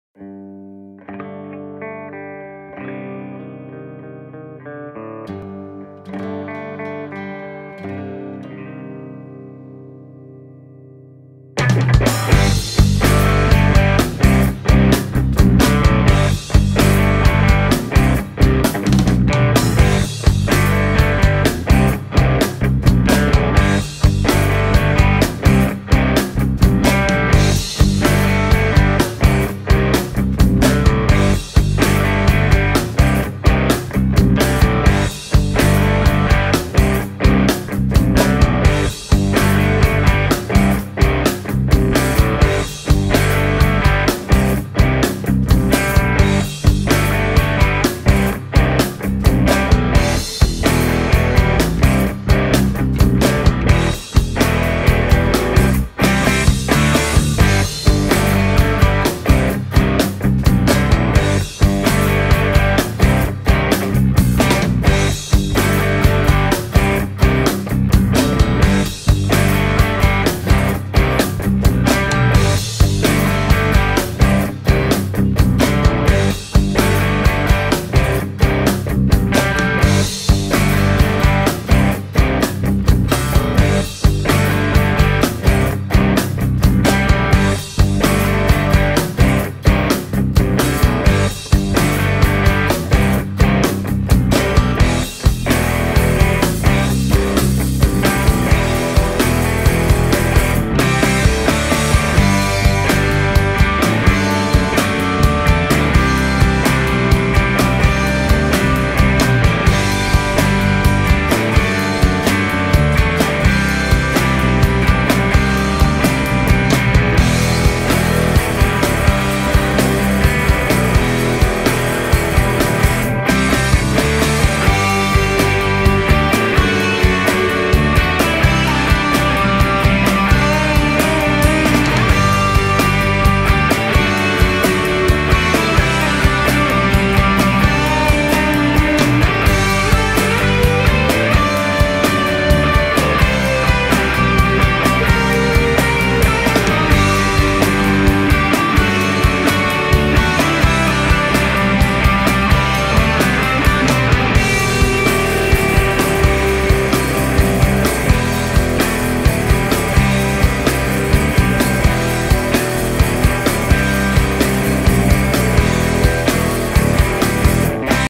BARCS Pet Portrait Sketches by Ania Milo
Sketches of pet portraits by Ania Milo; music by Meta Sun Kings